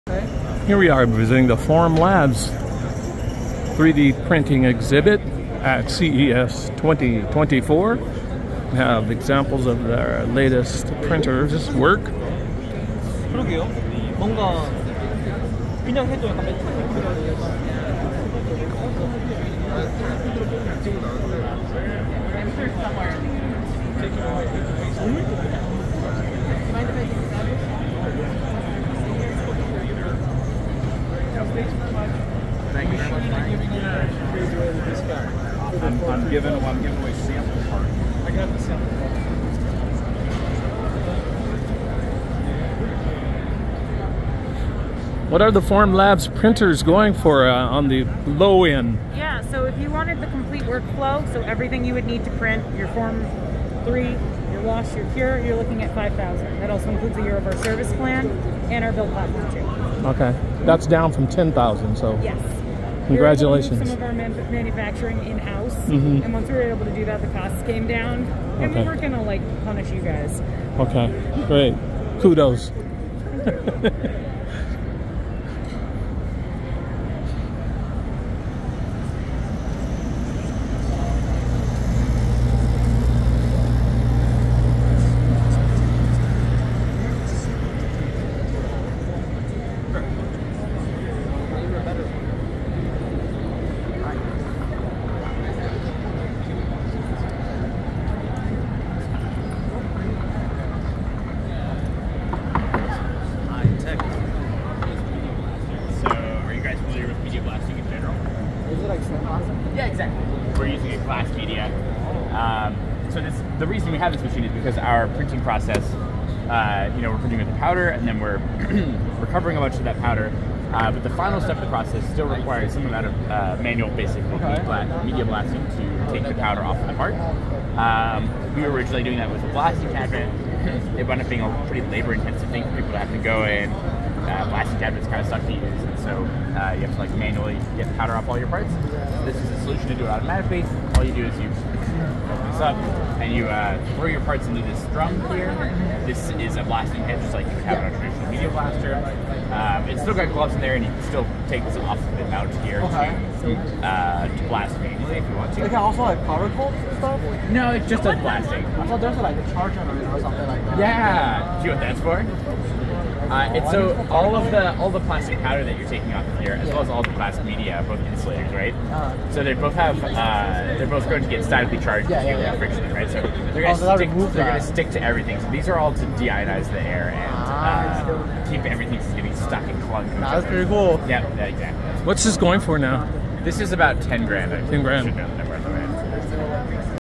New Formula Labs 3D Printers are Here
Formula Labs Booth at 2024 CES in Vegas was well received. They have new tech at reduced price point. Thank you wonderful people!